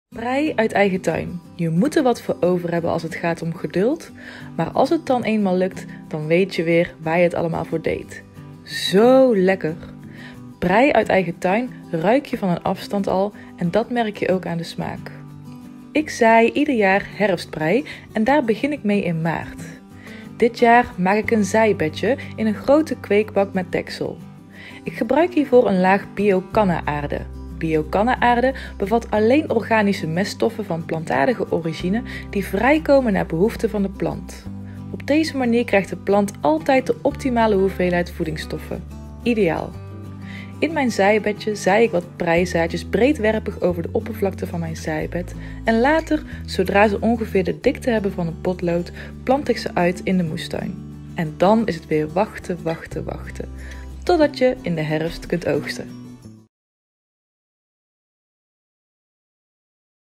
Stadstuinieren - Prei zaaien
Een groente die niet mag ontbreken in je eigen (moes)tuin…Prei! Wij laten zien hoe je prei zaait 🌱